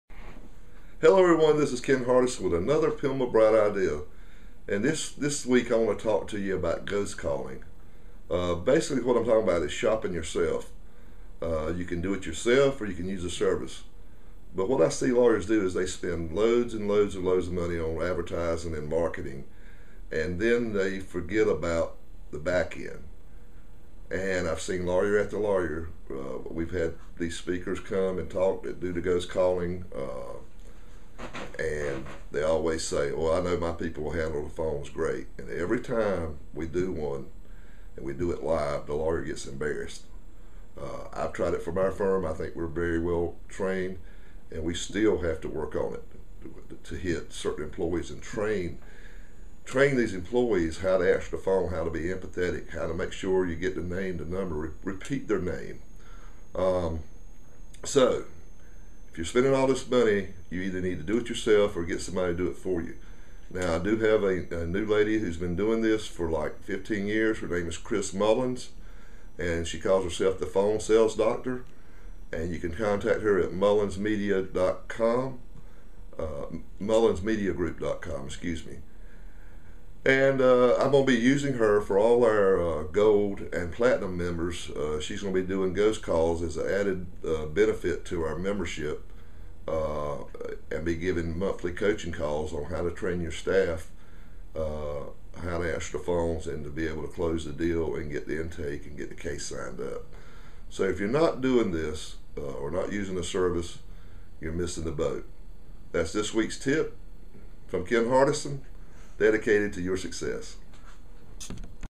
Ghost Calling: Not Just for Halloween - Law Firm Management
Ken Hardison, President of PILMMA, explains why you need to use ghost calls in your Law Firm to ensure your staff are answering the phone and handling intake calls correctly